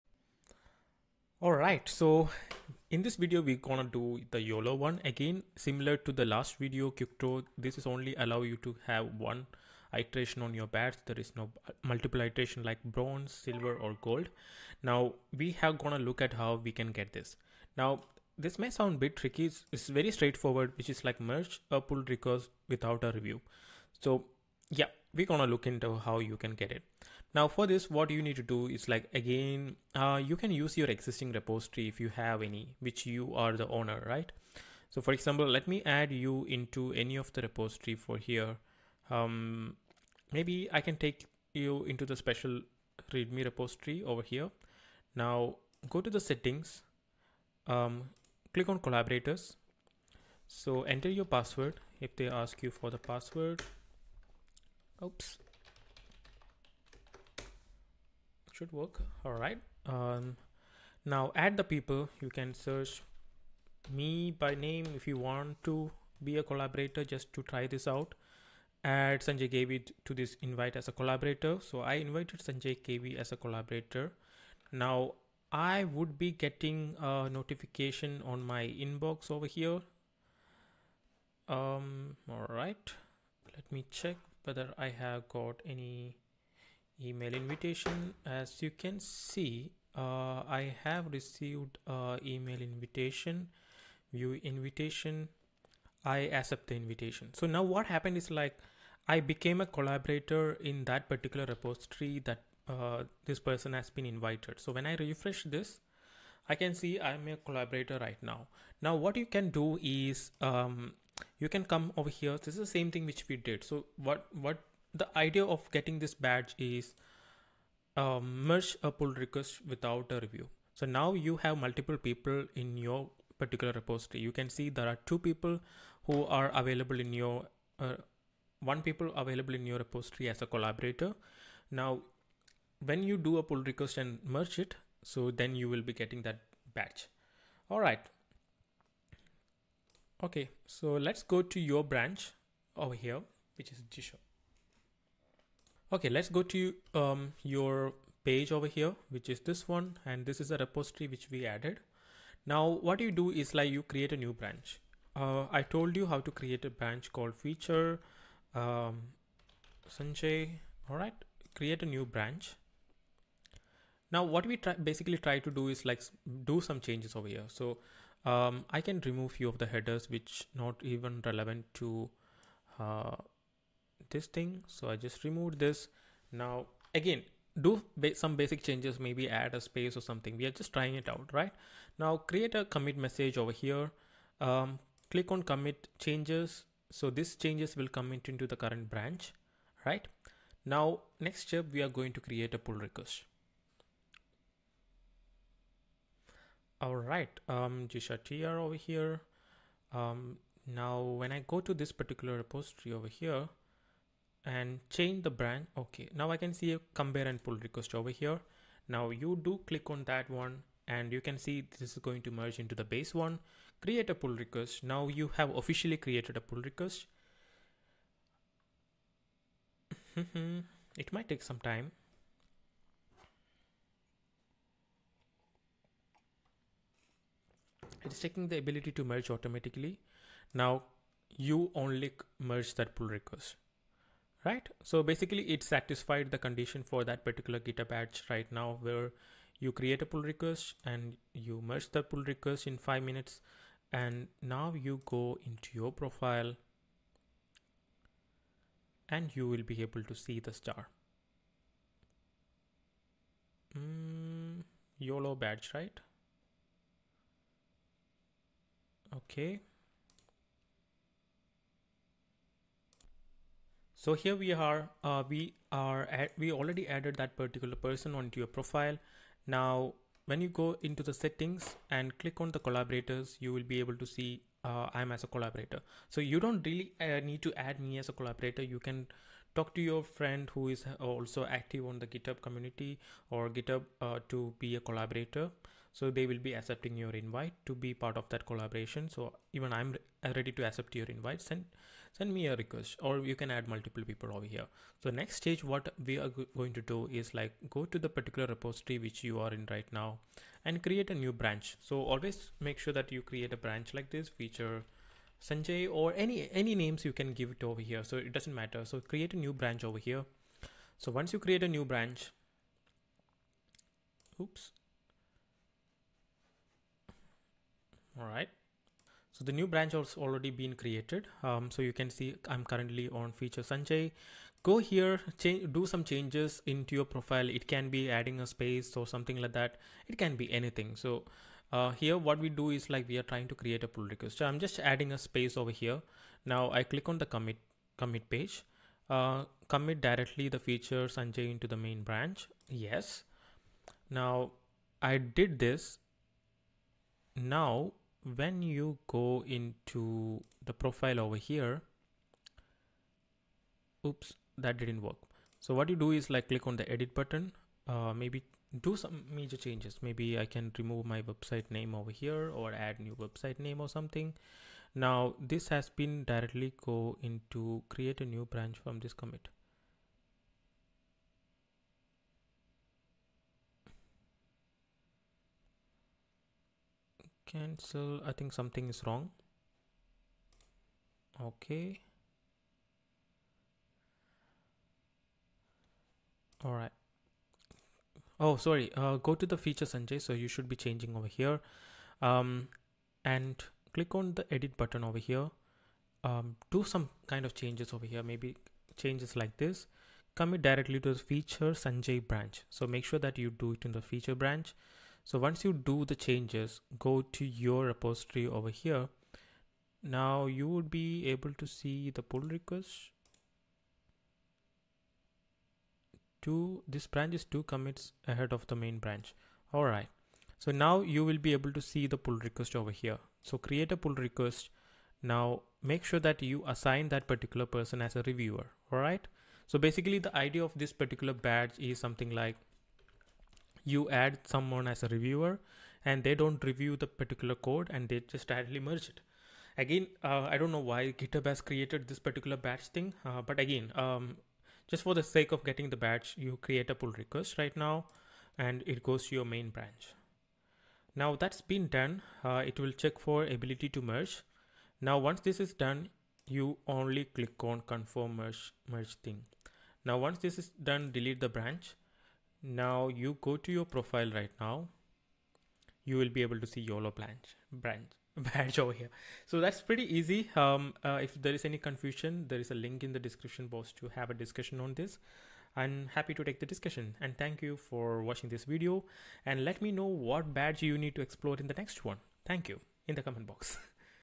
How to get Yolo badge on GitHub
Learn how to earn the GitHub Galaxy Yolo badge by participating in discussions and answering questions correctly.

Learning objectives 🔍
In this module, you'll: 👇🏻
🔸Explanation of the Yolo GitHub badge and its requirements.
🔸How to add a collaborator to your GitHub repository.
🔸Steps to create a new branch in GitHub
🔸Creating a pull request for the changes made in the branch.
🔸Completing the process and confirming that the Yolo badge is earned.

⌛Timestamps:
0:05 -  Introduction & Overview
0:28 -  Inviting a Collaborator
20:00 - Creating a Branch & Making Changes
3:26 - Creating & Merging a Pull Request
7:19 - Final Badge Confirmation & Closing

🔥 Ready for a deep dive?

Recode Hive– Helping You To Learn Developing with Less Coding to Make a Better Tomorrow.